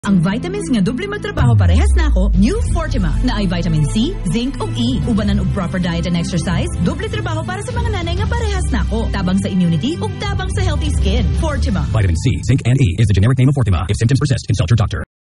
Regional Radio Ads: Fortima Radio Commercial 2H 2021 15s (in Cebuano)
This Regional Radio Commercial was sourced from DXBM-FM 90.7 MHz Love Radio Davao.

DRPS Two Episode No. 760: Regional Radio Ads: Fortima Radio Commercial 2H 2021 15s (in Cebuano)